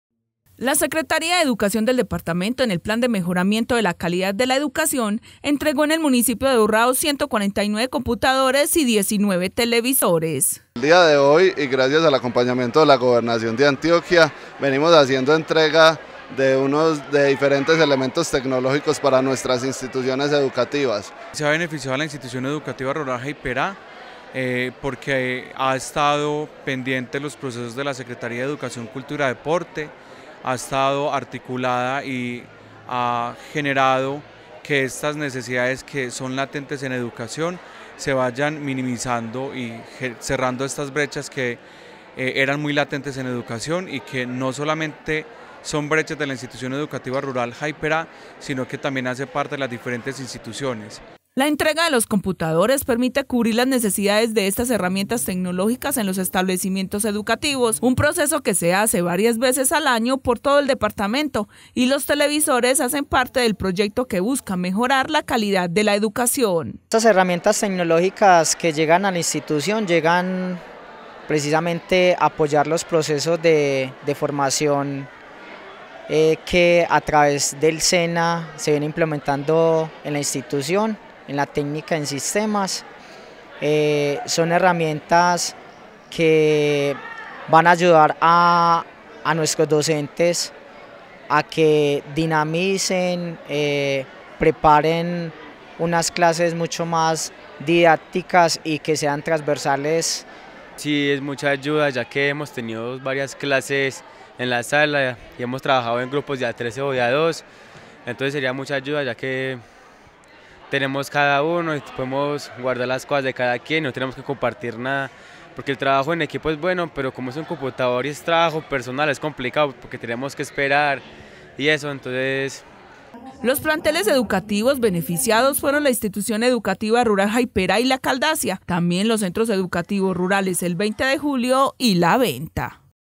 Equipos para mejorar la calidad educativa
Iniciamos el ciclo educativo con buenas noticias para las instituciones educativas, que recibieron equipos de cómputo y televisores por parte de la Secretaría de Educación de Antioquia, bajo el proceso de Mejoramiento en la Calidad de la Educación.